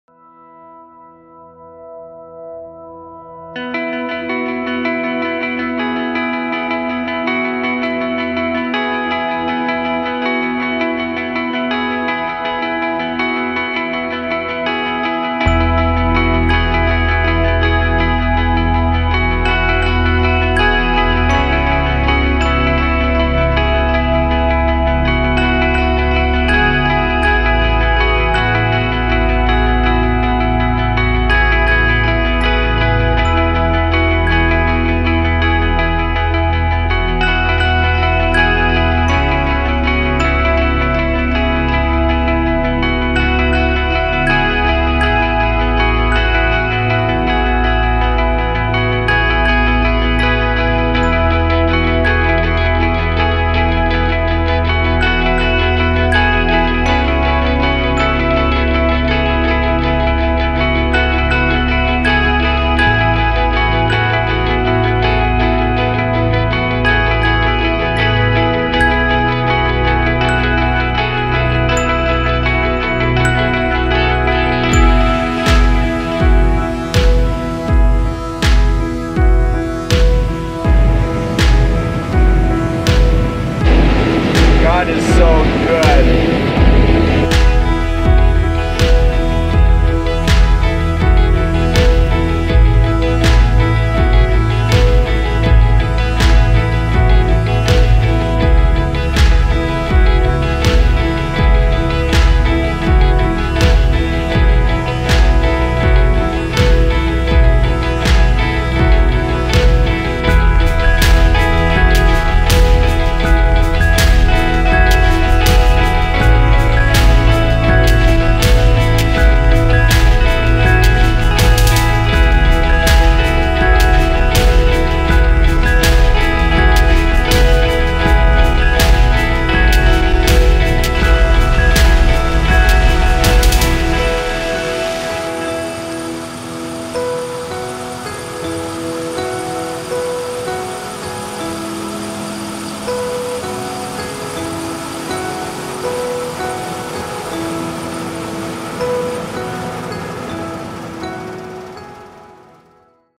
John Deere S680 Combine and 18-Row Head in Action - Peterson Corn Harvest November 2016
The Corn Harvest of 2016.  Some amazing Yields this year.  Check out the John Deere S380 with the 18-Row Head in action.